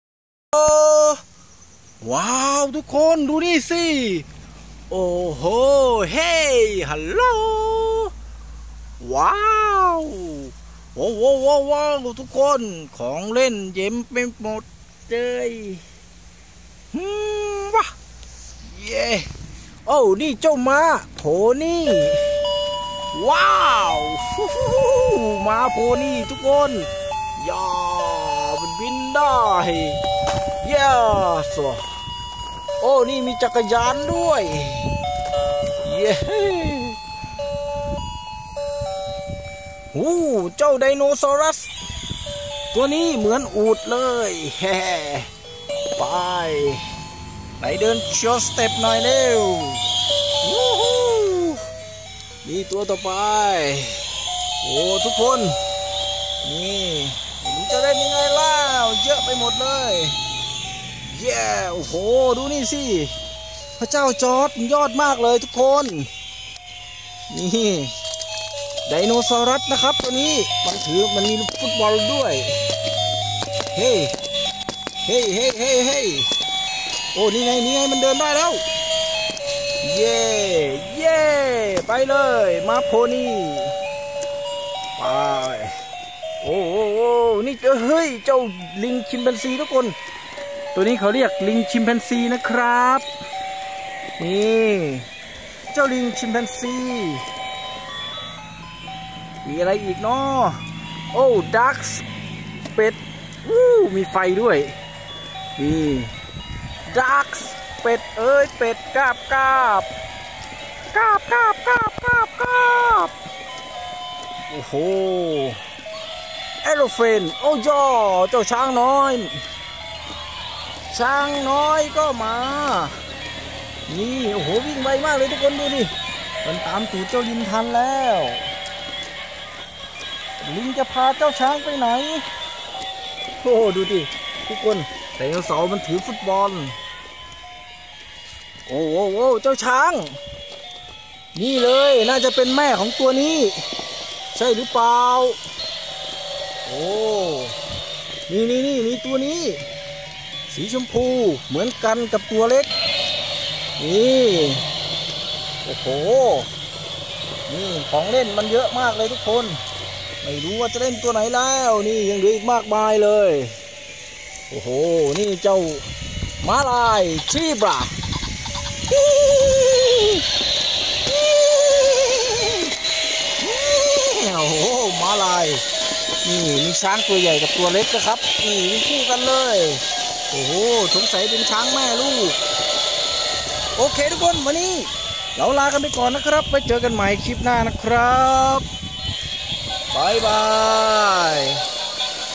ของเล่นมากมายมีทั้ง ไดโนเสาร์ คิงคองยักษ์ ช้าง วัว ปลา เป็ดก้าบก้าบ ควาย รถก่อสร้าง
Lots of toys including dinosaurs, giant King Kong, elephants, cows, fish, clawed ducks, buffaloes, construction vehicles.